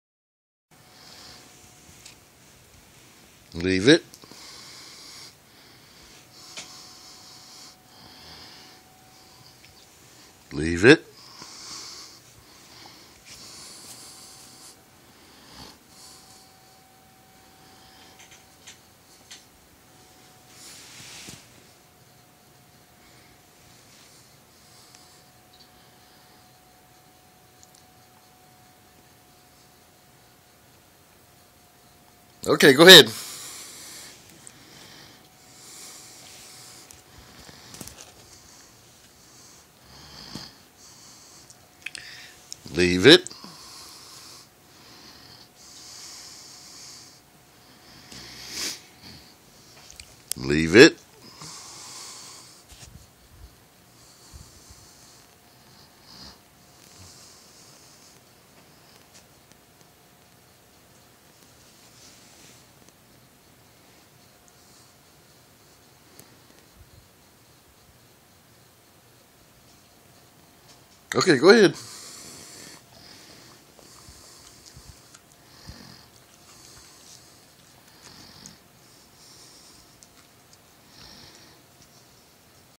Sheltie Playing "Leave It"
Working with Lea ad Luke on the "Leave It" command. Lea is so cute, looking up to make sure she "leaves it", while Luke has to look away. As if, if they see the treat they will just have to eat it. Follow their life with us in the following thread.